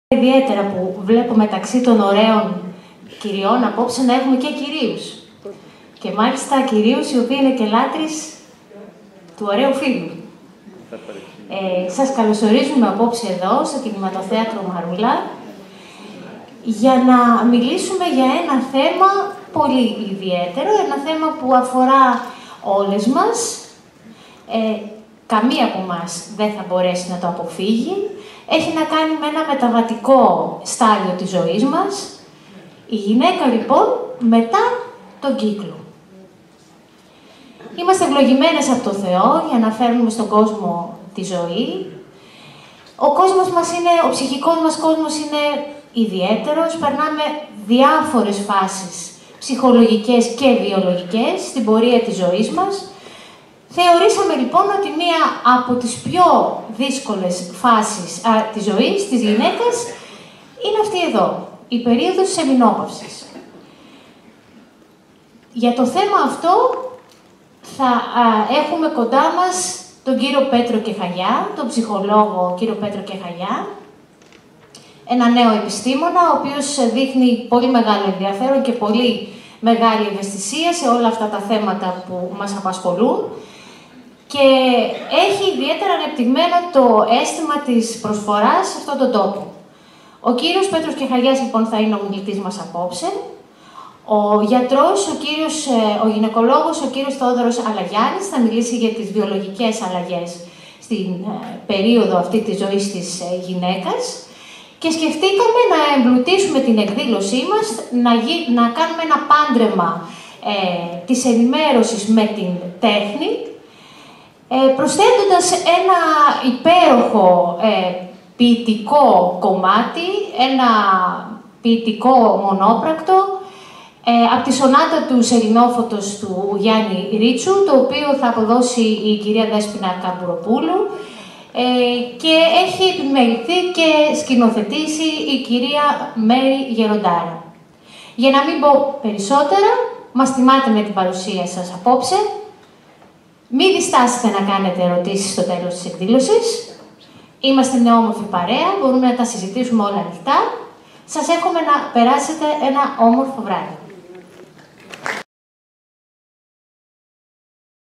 LimnosReport web tv: Η γυναικά μετά τον κύκλο. Πρόεδρος Δημοτικής Κοινότητας Μύρινας
Την Δευτέρα  19 Μαρτίου 2018  η Δημοτική Κοινότητα Μύρινας  πραγματοποίησε  στο Κινηματοθέατρο «ΜΑΡΟΥΛΑ» εκδήλωση (ενημερωτική-επιστημονική ομιλία) με θέμα «Η  γυναικά μετά τον κύκλο»  με ομιλητές τον Νευροψυχολόγο-Ψυχοθεραπευτή κο Πέτρο Κεχαγιά και τον Γυναικολόγο/Μαιευτήρα κ. Θεόδωρου Αλαγιάννη.
Καλοσόρισμα της Προέδρου της Δημοτικής Κοινότητας Μύρινας Αδαμαντία Γαροφαλλίδου-Παπούλκα